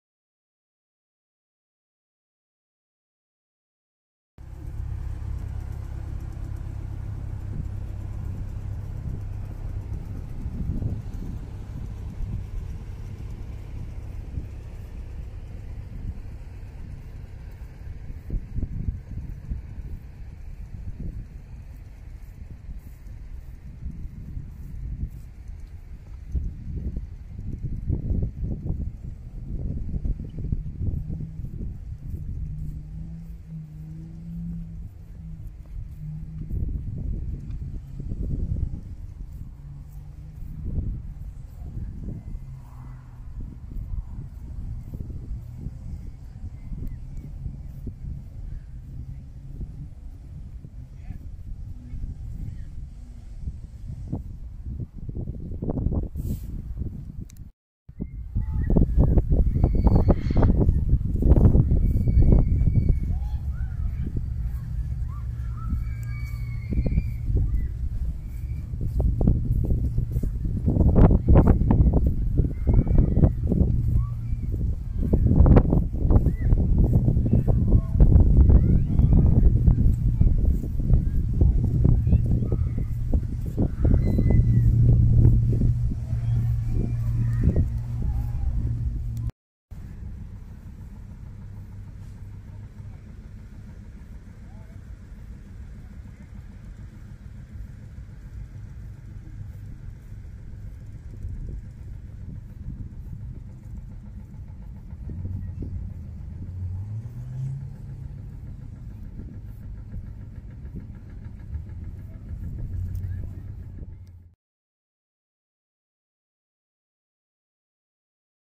***Raw footage*** Whitby steam rally (commercials)
Whitby steam rally 2021

This is a very short video of the commercials at whitby steam rally.

Videos taken on a huawei p 30 pro.